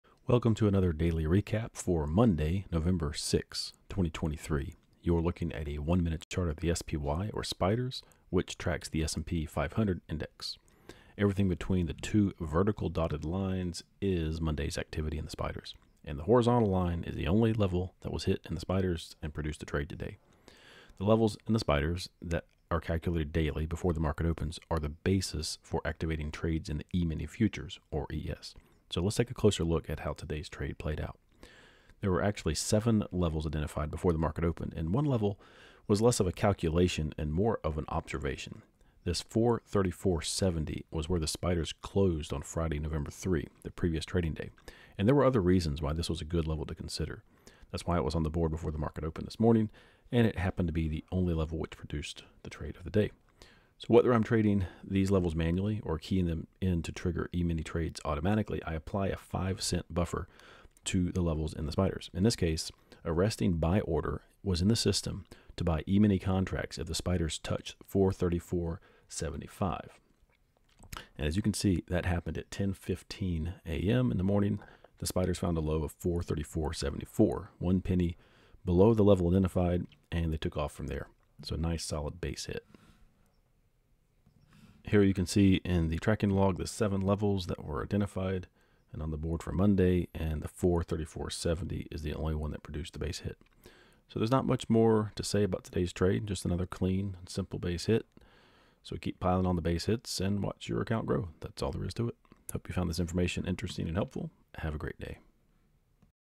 One Picture-Perfect Trade for Multiple ES Points [Never Out of the Money]
SPY / E-Mini futures trading recap for November 6, 2023.